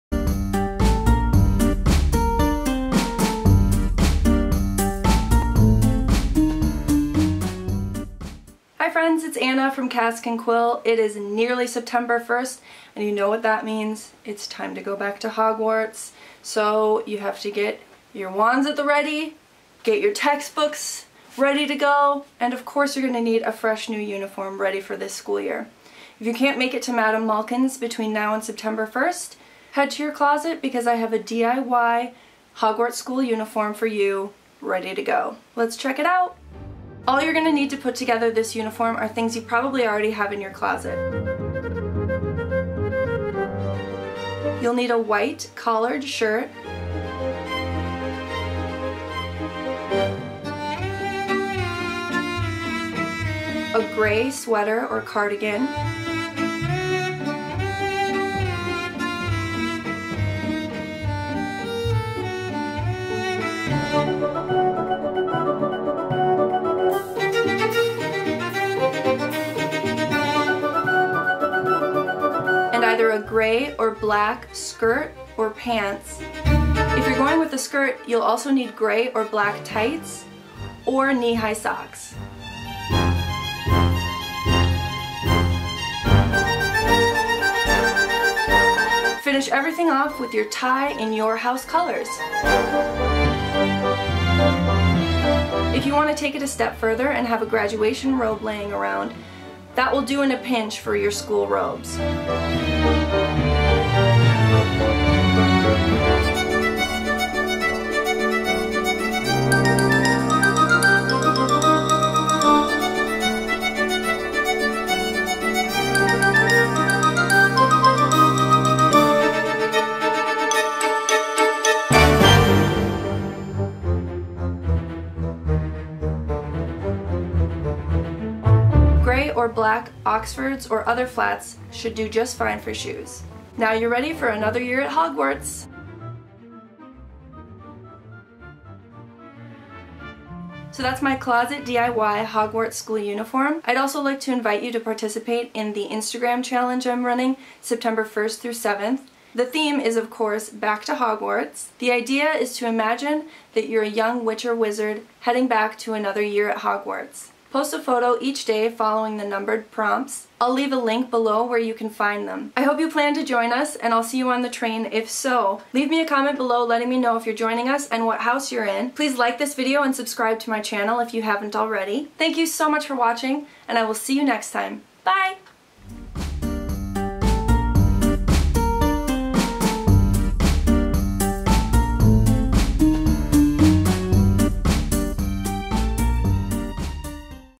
DIY Hogwarts School Uniform
It's time to go back to Hogwarts! Find the materials for your uniform right in your closet. :)